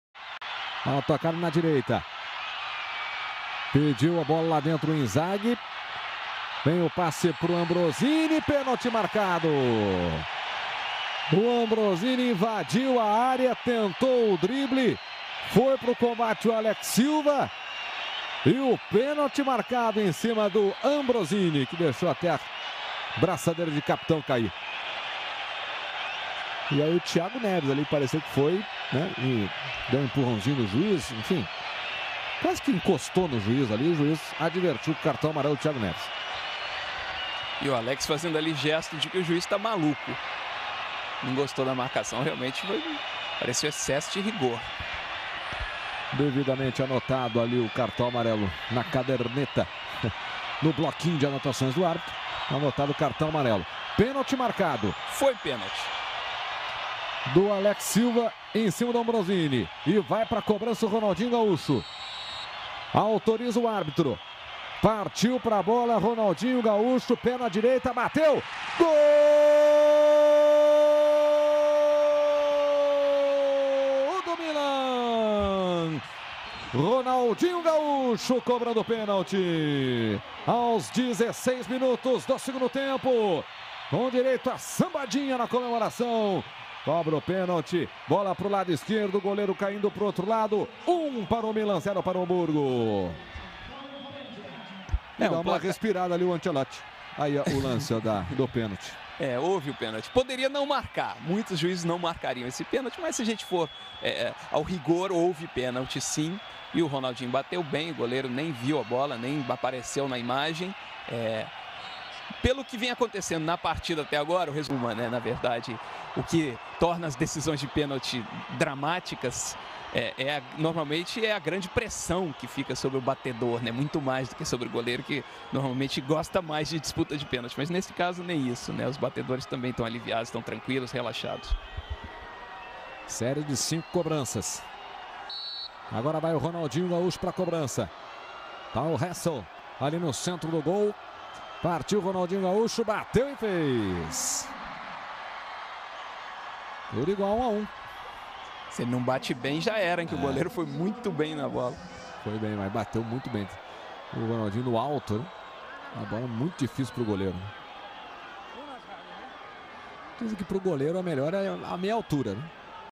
Ronaldinho Gaúcho (Milan) - 06/01/2009 - Milan 1(5)x(4)1 Hamburgo-ALE - 1 gol (1p)
Torneio de Dubai 2009